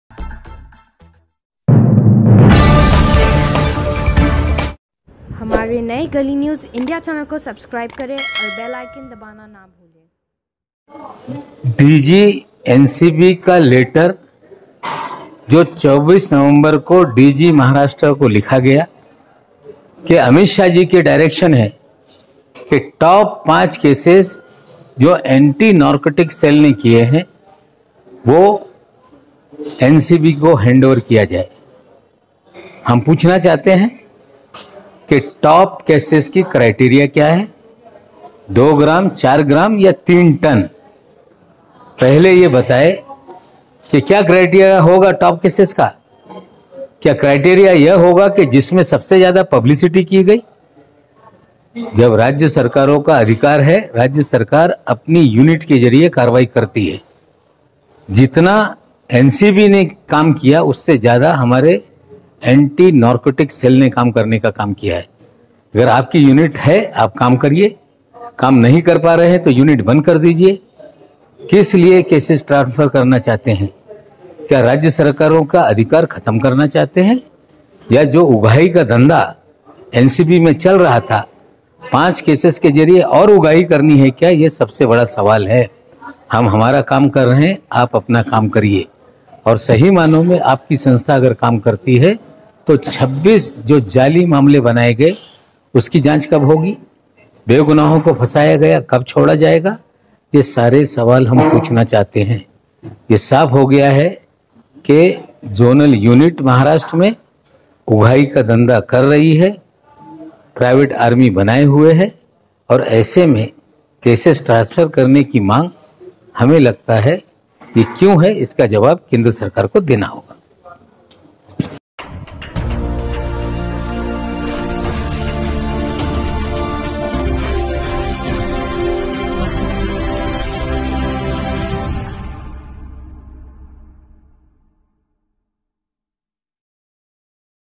Amit Shah directed ANC 5 Top cases NCB ke hand over kare Nawab Malik ne uthaye sawal
Amit Shah directed ANC 5 Top cases NCB ko hand over kare Nawab Malik ne uthaye sawal

Maharashtra Minister Nawab Malik ne Home Minister Amit Shah ke diye hue letter par sawal uthaye jisme Maharashtra ANC ko 5 Top Most Cases NCB ko hand over karne kaha gaya hai

Nawab Malik ka Amit Shah ko sawal hai ke ANC ke Top 5 cases ka Criteria kiya hai 2 gram, 4 gram, ya 3 Tonnes quantity wala case ?